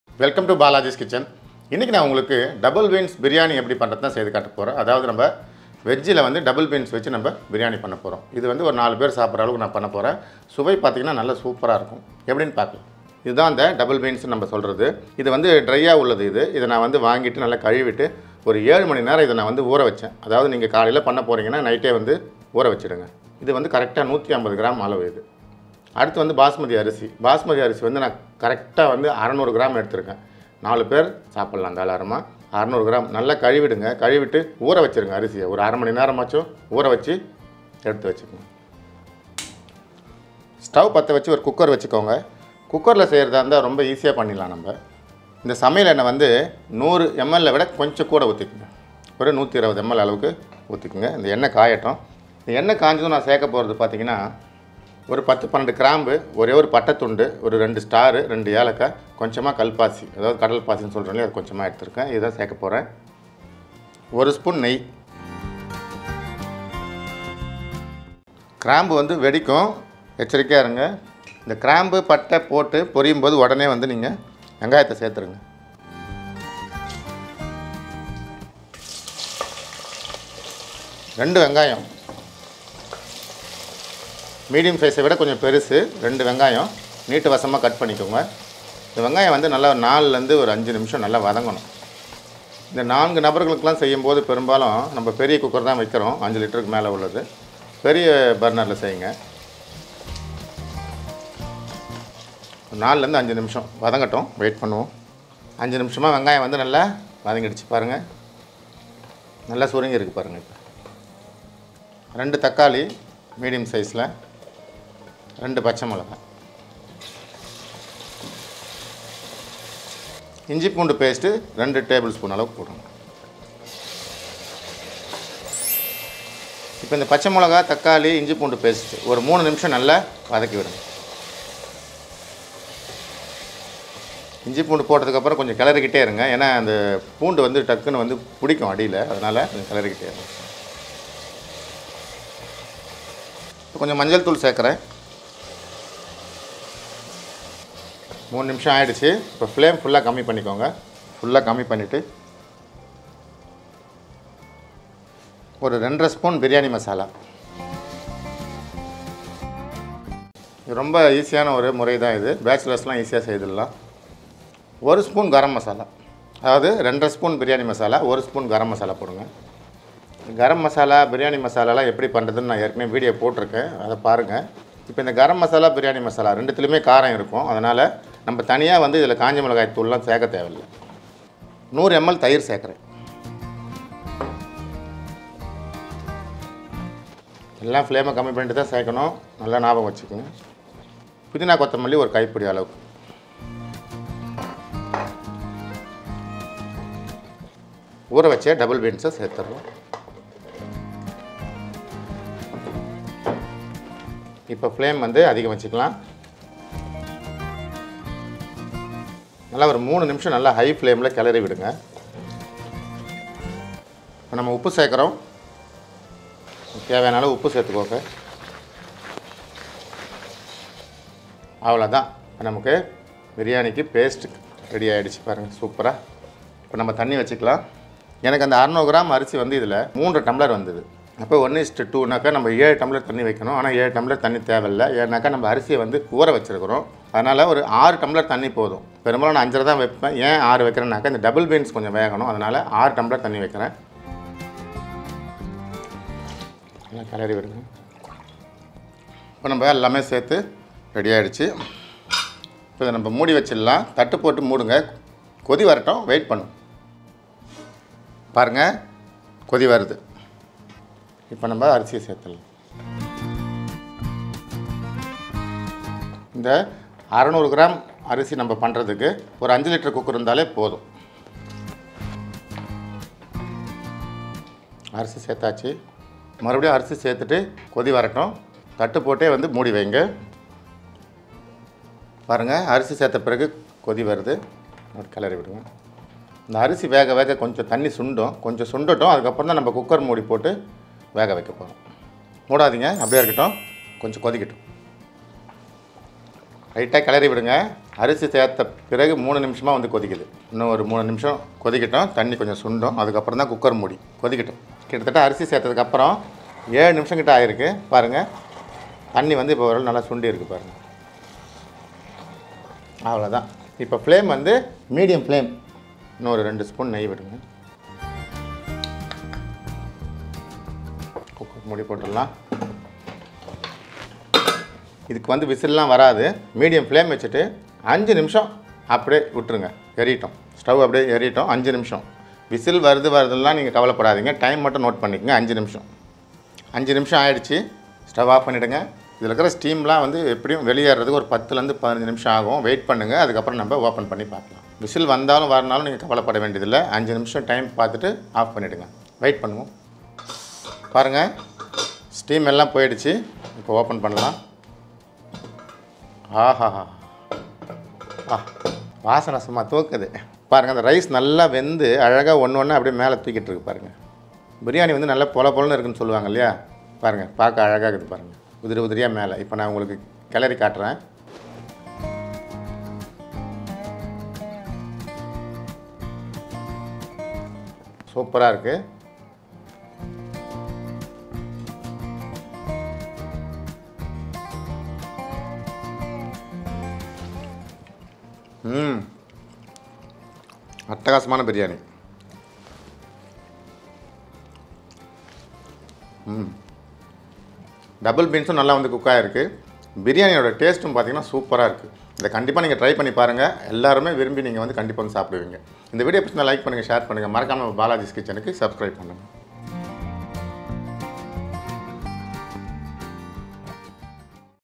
குக்கரில் டபுள் பீன்ஸ் பிரியாணி செய்வது எப்படி | Double beans biryani | Balaji's kitchen
Double beans biryani | டபுள் பீன்ஸ் பிரியாணி  | Veg Biryani recipe

சுவையான டபுள் பீன்ஸ் பிரியாணி குக்கரில் எப்படி செய்வது என்பது பற்றி இந்த வீடியோ பதிவில் காண்போம்

INGREDIENTS :
• 150g Double beans ( soaked )
• 600g Basmati rice ( soaked )
• 120ml Cooking oil
• 10 Clove
• 1 Cinnamon
• 2 Cardamom
• 2 Star anise
• Kalpasi
• 3tsp Ghee
• 2 Onion
• 2 Tomato
• 2 Green chilli
• 2tbsp Ginger garlic paste
• Turmeric powder
• 2.5tsp Biryani masala
• 1tsp Garam masala
• 100ml Curd
• Mint leaves
• Coriander leaves
• 6tblr water